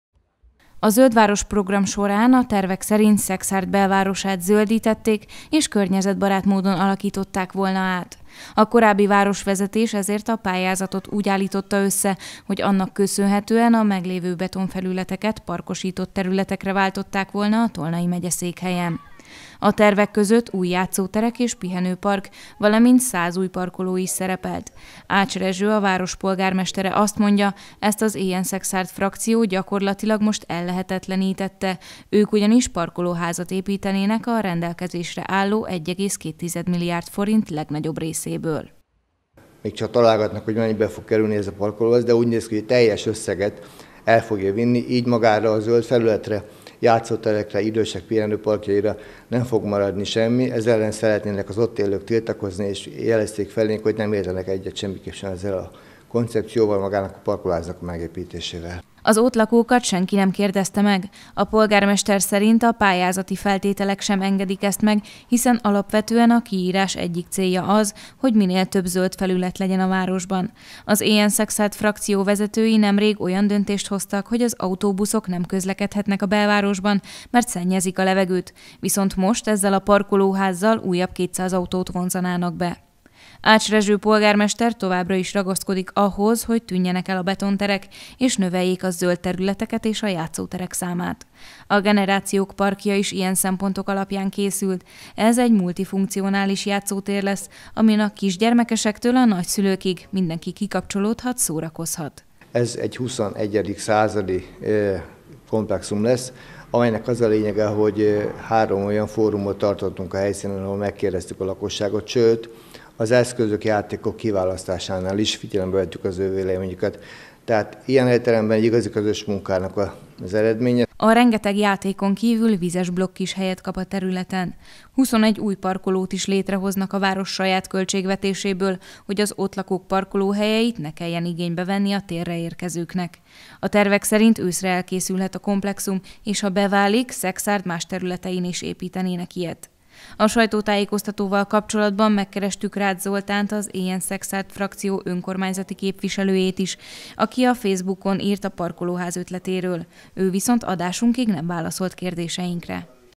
Parkolóházat építtetne zöldítés helyett az Éljen Szekszárd frakció a polgármester szerint
Megérkezett az első lakossági tiltakozás a parkolóház ellen, amit az Éljen Szekszárd frakció építtetne a város központjába – erről Ács Rezső, a város polgármestere beszélt hétértékelő sajtótájékoztatóján. A beruházást az Éljen Szekszárd képviselői a Zöld Város program részeként valósítanák meg. Az projekt eredeti terveiben nem szereplő beruházás viszont szinte a teljes pályázati összeget felemésztené, így nem maradna pénz például zöldítésre.